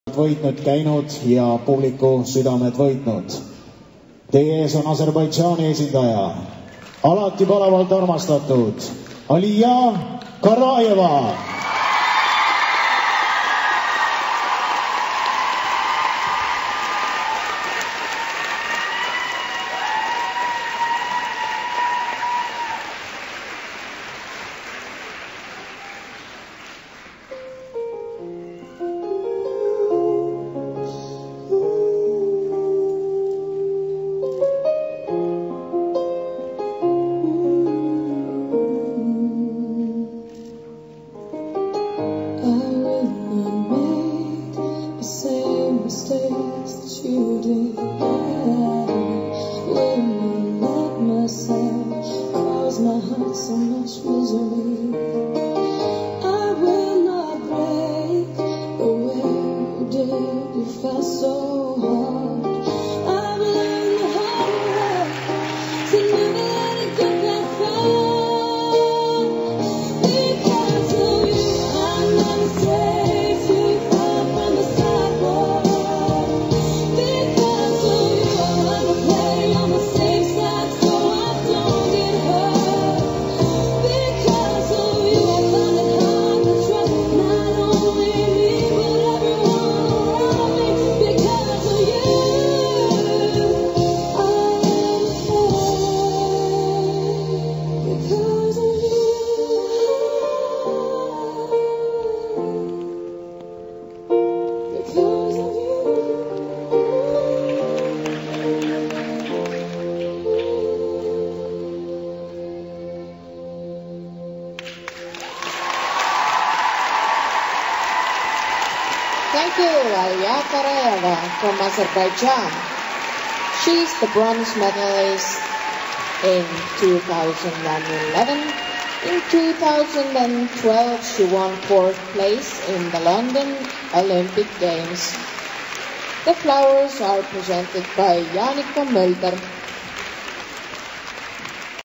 Aliya Garayeva 2013 Miss Valentine Gala
February 10th of the 2013 Tartu (Miss Valentine) World Cup

Video is credited to Marc Schuirmann